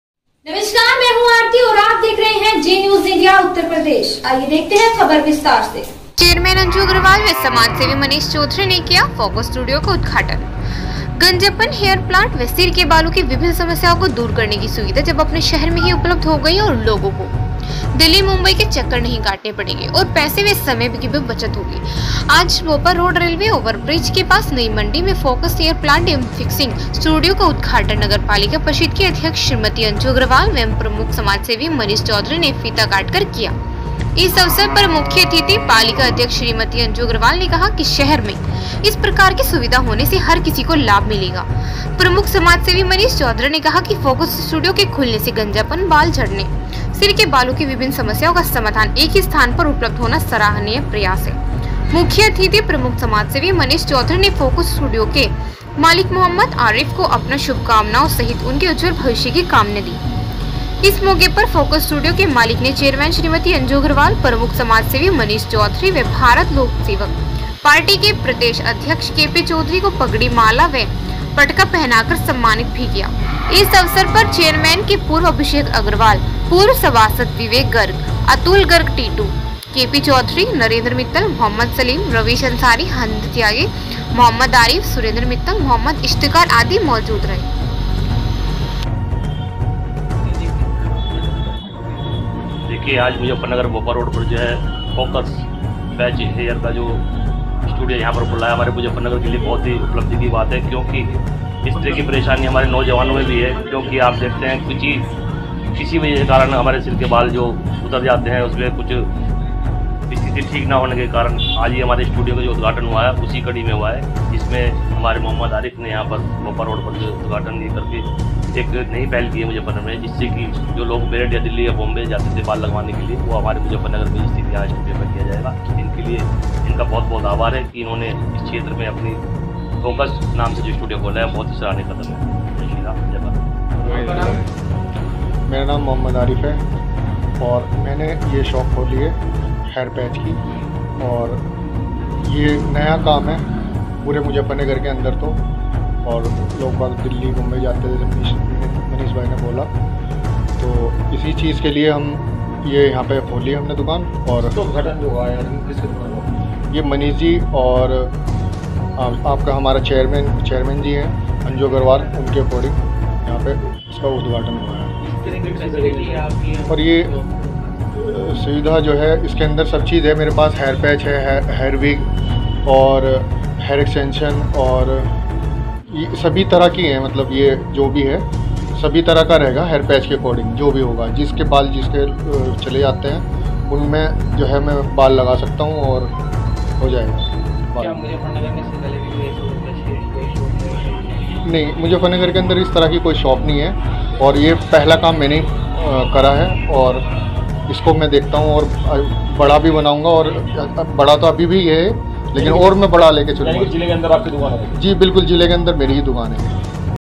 चेयरमैन अंजू अग्रवाल व समाजसेवी मनीष चौधरी ने किया फोकस स्टूडियो का उद्घाटन
चेयरमैन अंजू अग्रवाल व समाजसेवी मनीष चौधरी ने किया फोकस स्टूडियो का उद्घाटन   मुजफ्फरनगर। गंजापन, हेयर प्लांट व सिर के बालों की विभिन्न समस्याओं को दूर करने की सुविधा अब अपने शहर में ही उपलब्ध हो गई और लोगों को दिल्ली, मुंबई के चक्कर नहीं काटने पडेंगे और पैसे व समय की बचत भी होगी। आज भोपा रोड रेलवे ओवर ब्रिज के पास, नई मंडी में फोकस हेयर प्लांट एंड फिक्सिंग स्टूडियो का उद्घाटन नगरपालिका परिषद की अध्यक्षा श्रीमती अंजू अग्रवाल व प्रमुख समाजसेवी मनीष चौधरी ने फीता काटकर किया। इस अवसर पर मुख्य अतिथि पालिका अध्यक्ष श्रीमती अंजू अग्रवाल ने कहा कि शहर में इस प्रकार की सुविधा होने से हर किसी को लाभ मिलेगा। प्रमुख समाजसेवी मनीष चौधरी ने कहा कि फोकस स्टूडियो के खुलने से गंजापन, बाल झडने, सिर के बालों की विभिन्न समस्याओं का समाधान एक ही स्थान पर उपलब्ध होना सराहनीय प्रयास है। मुख्य अतिथि प्रमुख समाजसेवी मनीष चौधरी ने फोकस स्टूडियो के मालिक मौहम्मद आरिफ को अपनी शुभकामनाओं सहित उनके उज्जवल भविष्य की कामना की। इस मौके पर फोकस स्टूडियो के मालिक ने चेयरमैन श्रीमती अंजू अग्रवाल, प्रमुख समाजसेवी मनीष चौधरी व भारत लोक सेवक पार्टी के प्रदेश अध्यक्ष के.पी. चौधरी को पगडी, माला व पटका पहनाकर सम्मानित भी किया। इस अवसर पर चेयरमैन के पुत्र अभिषेक अग्रवाल, पूर्व सभासद विवेक गर्ग, अतुल गर्ग टीटू, केपी चौधरी, नरेंद्र मित्तल, मौहम्मद सलीम, रविश अंसारी, हरेंद्र त्यागी, मौहम्मद आरिफ, सुरेंद्र मित्तल, मौहम्मद इश्तकार आदि मौजूद रहे।